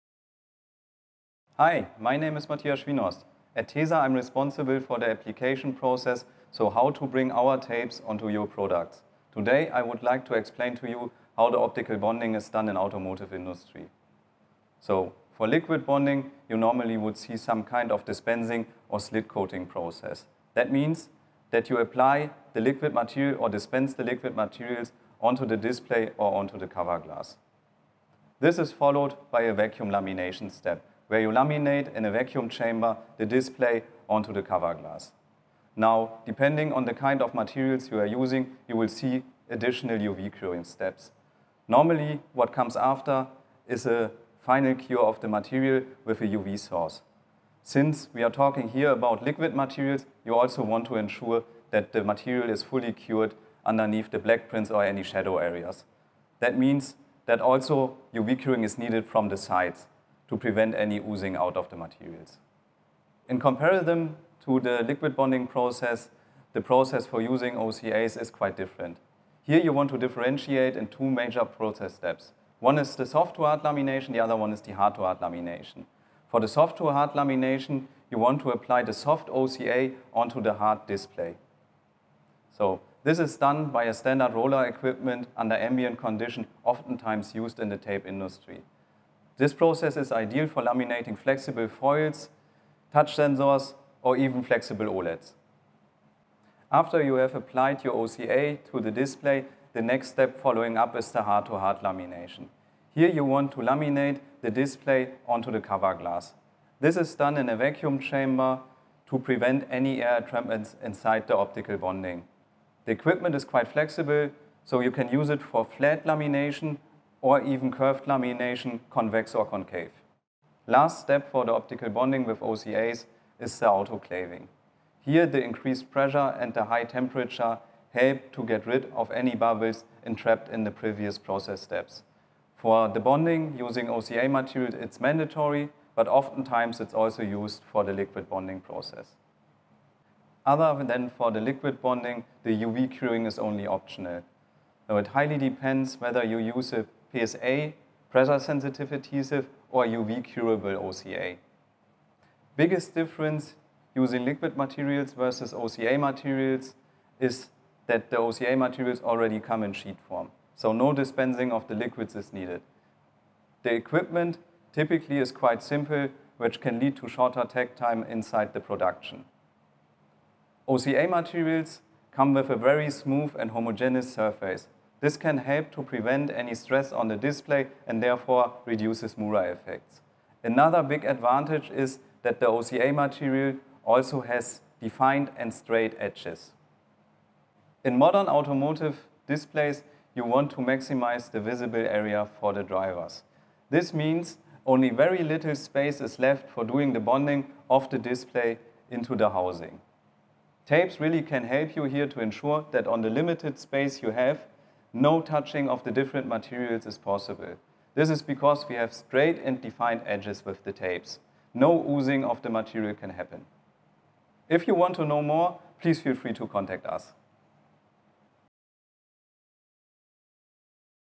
OCA lamination process: Liquid bonding vs. OCA lamination | tesa
In comparison to the liquid bonding process, the process for using OCA's is quite different. Here you want to differentiate in two major process steps. One is the soft to hard lamination. The other one is the hard-to-hard lamination.